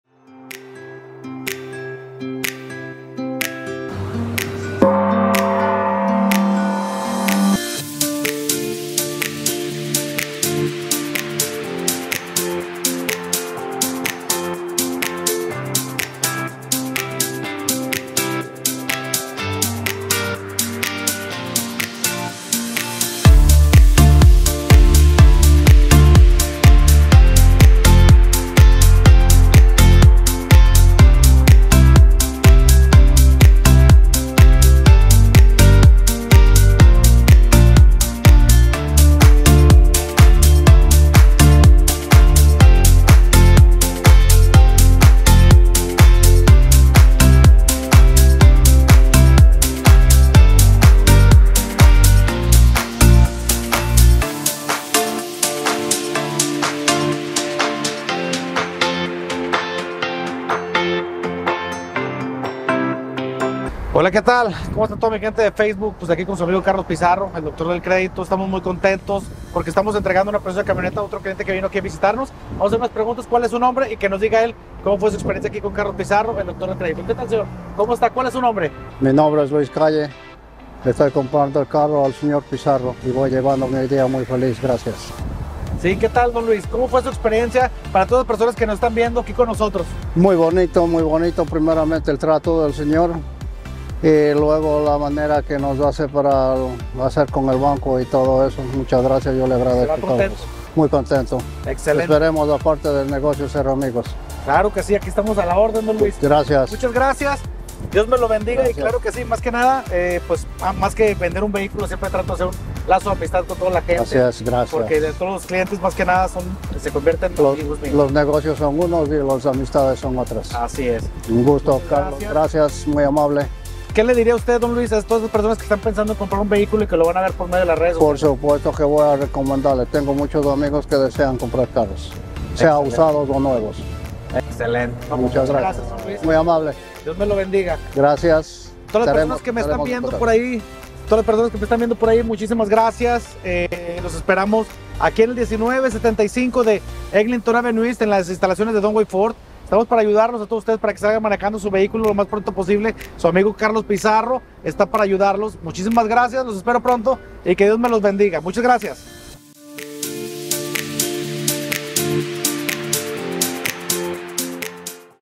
Muy buen trato - 2019 Range Rover Evoque - Doctor del Credito - Toronto - Canada.
Ven por tu 2️⃣0️⃣1️⃣9️⃣ Range Rover Evoque 🚙

Muy bonita experiencia, el trato de Carlos Pizarro el doctor del credito muy bueno, me voy muy contento por la ayuda en la aprobacion con el banco, espero que aparte de hacer negocios poder ser amigos, muchas gracias por la ayuda. 🇲🇽📍🇨🇦